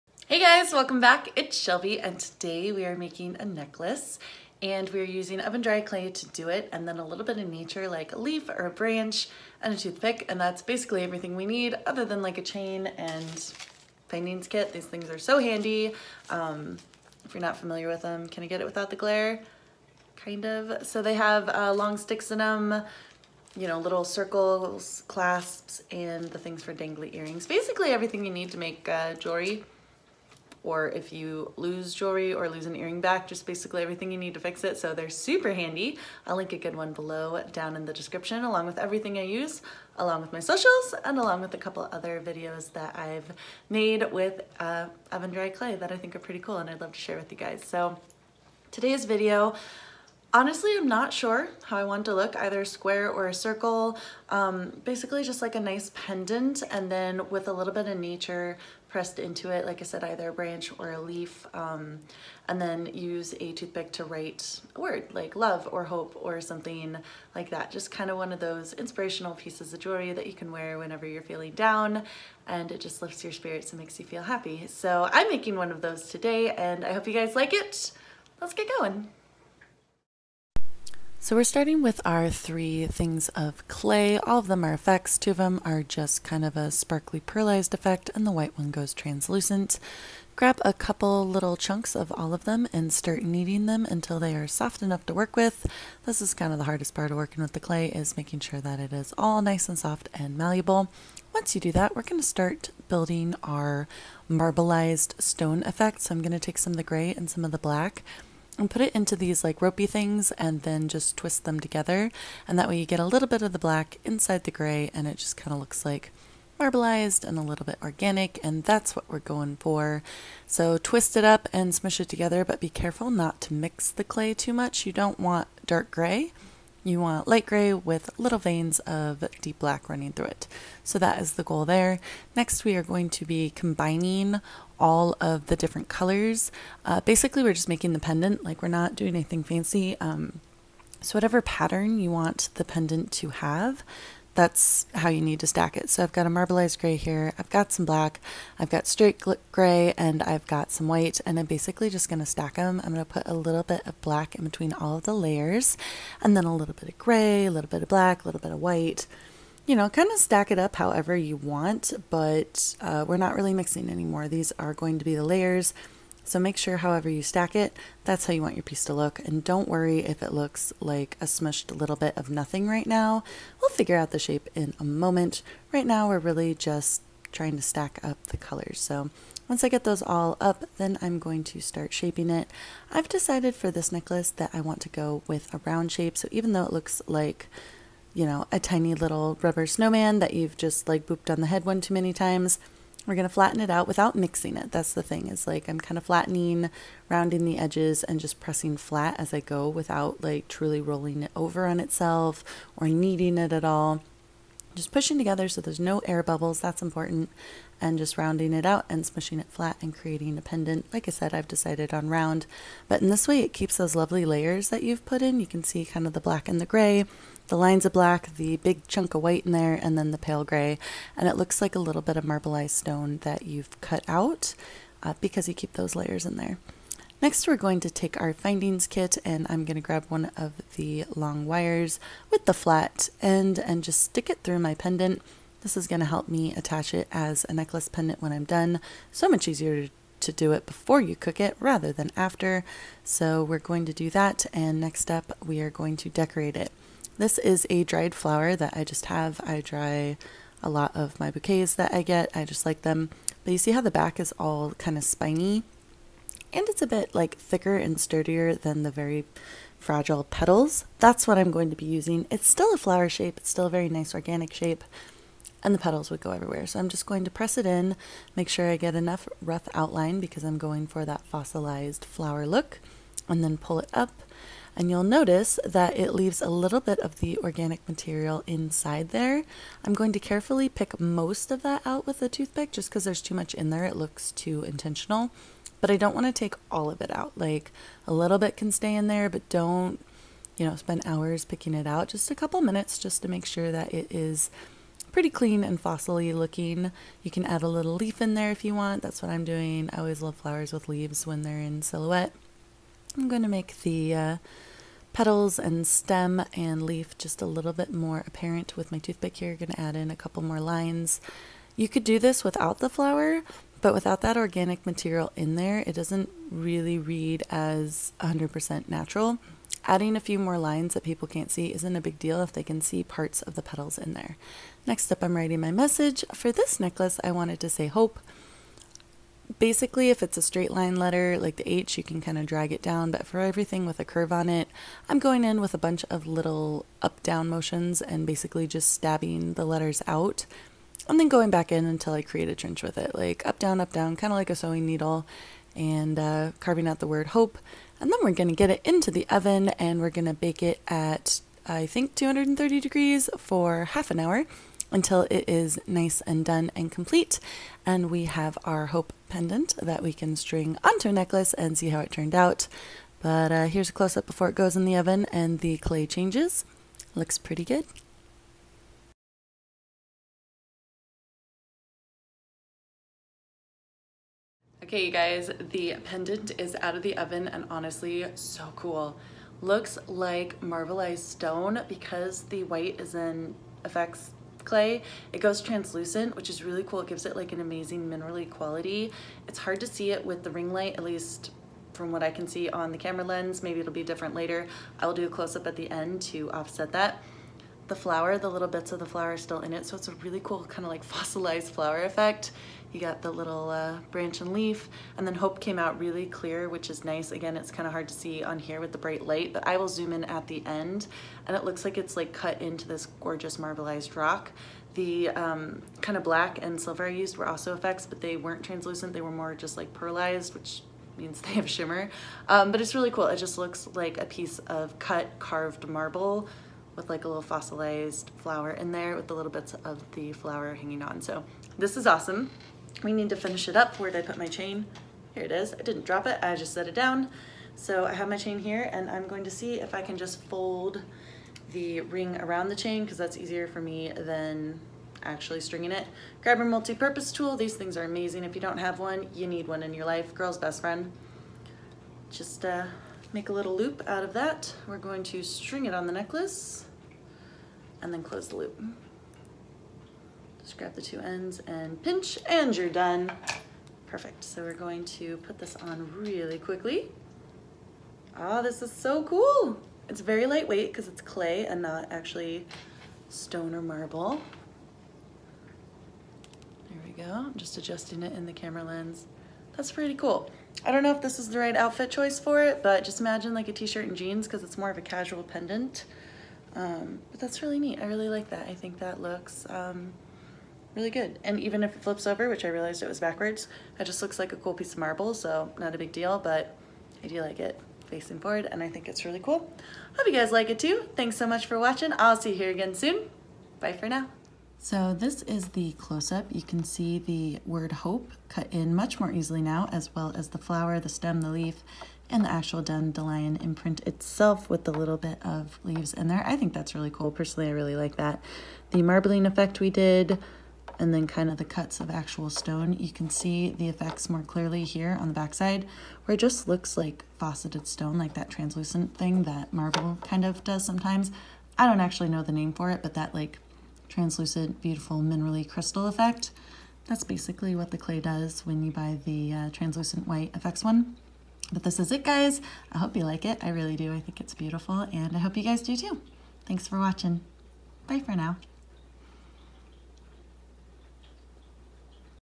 DIY Marble Hope Pendant
Hi Everyone!

Today we’re using oven dry clay, an findings kit, and a dried flower to make an adorable inspirational pendant for your necklace. What I love about this DIY is that it’s a simple idea turned into a fabulous piece of jewelry. The marble effect of the clay in this craft adds a glamorous air of sophistication. While the faux fossilized flower imprint lends an interesting whimsy, and the carved in word ‘hope’ ties it all together by expressing a joyful and optimistic statement that is both a reminder to the wearer and also to the viewer.

This video isn’t meant to be a full tutorial, rather, it’s just my experience making this item. But I think this is a great piece to keep or gift to a friend. I hope you like it.

If you’d like to see a few of my other clay videos I’ll link them here:

If you’re looking to make your own and are interested in the products I used they are linked below.

Multipurpose Tool
FIMO Oven-Dry Clay Set
Findings Kit

Happy Crafting!